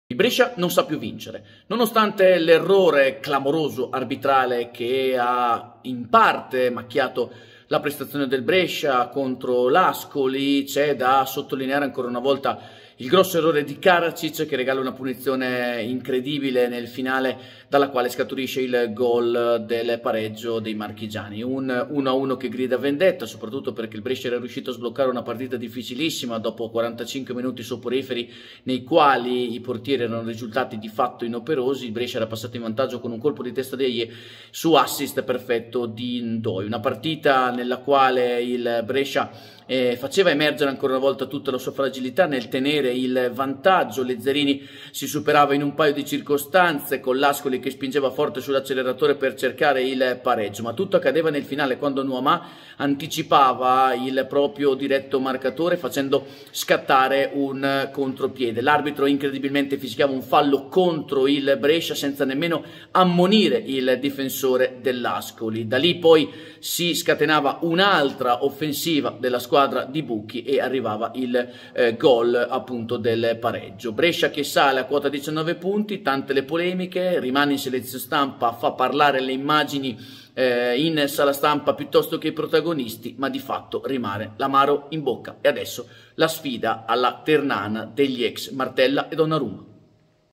Brescia-Ascoli 1-1: l'analisi di Fabio Pettenò
Pareggio tra le polemiche per il Brescia contro l'Ascoli. L'analisi di Fabio Pettenò  dopo la gara del Rigamonti